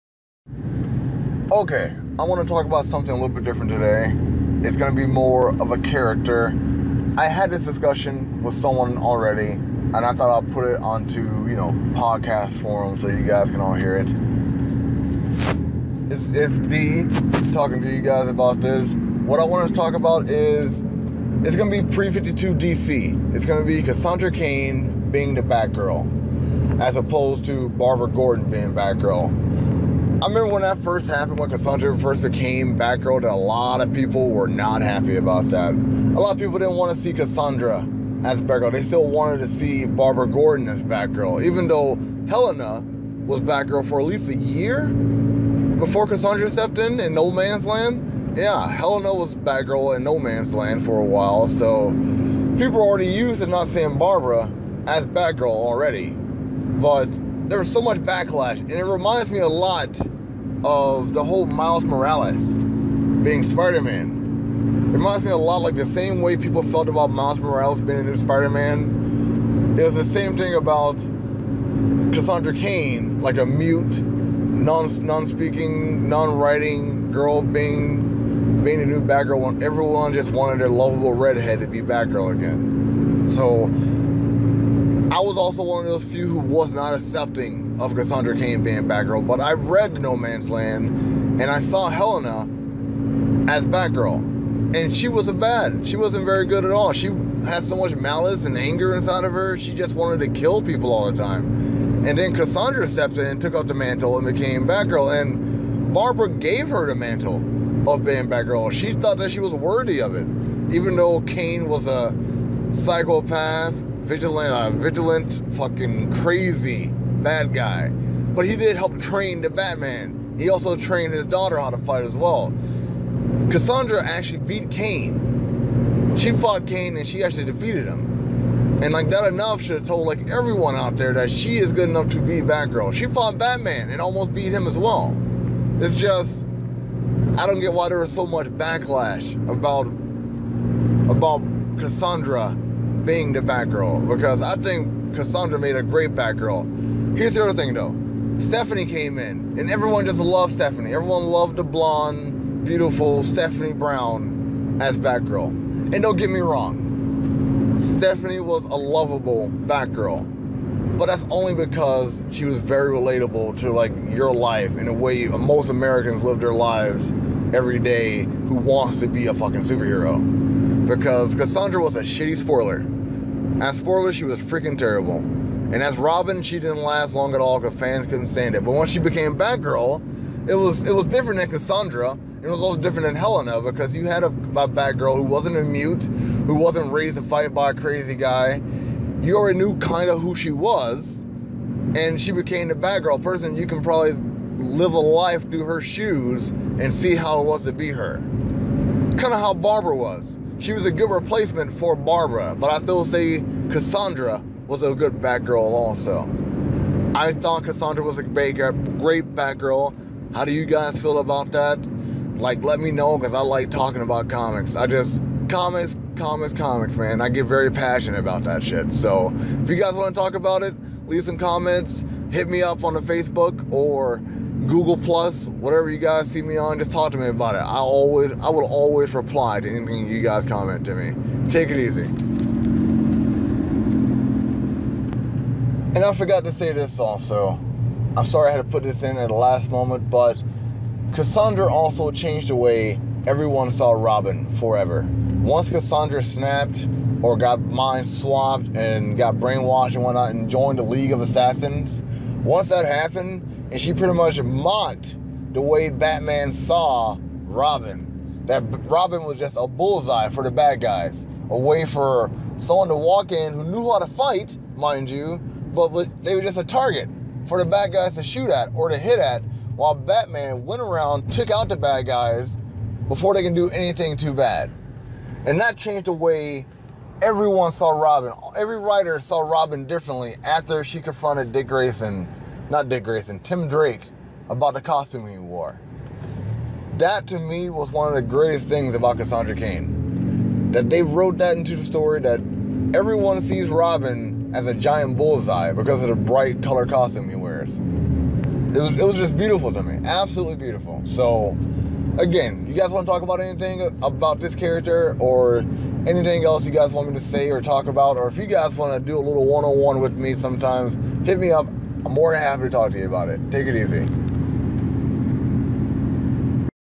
Comic Talk: Batgirl
Just me talking about batgirl and dc comics